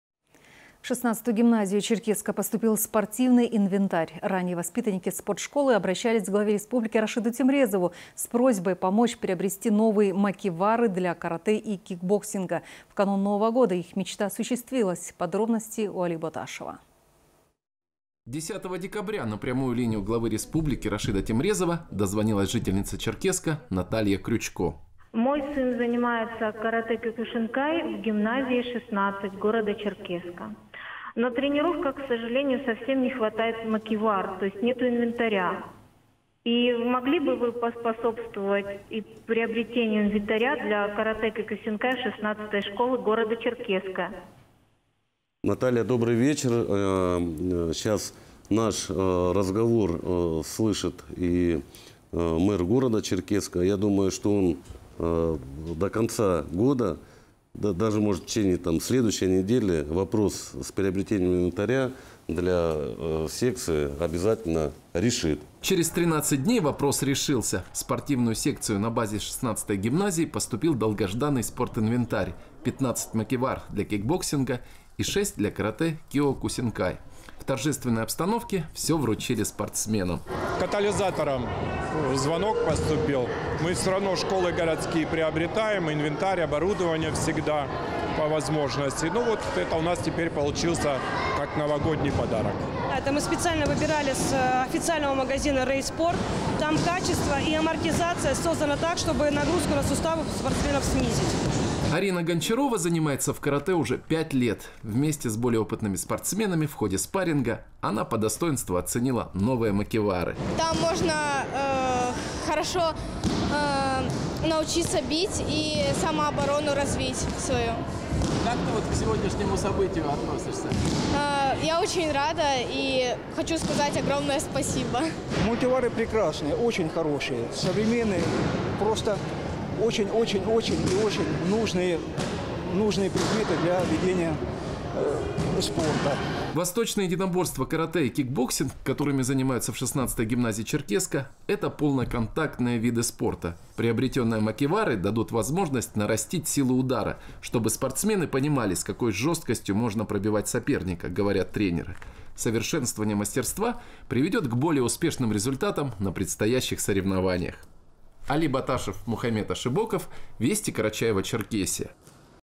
В 16-ю гимназию Черкесска поступил новый спортивный инвентарь
В 16-ю гимназию Черкесска поступил спортивный инвентарь. Ранее, воспитанники спортшколы обратились к Рашиду Темрезову, с просьбой помочь приобрести новые макивары для карате и кикбоксинга. В канун Нового года их мечта осуществилась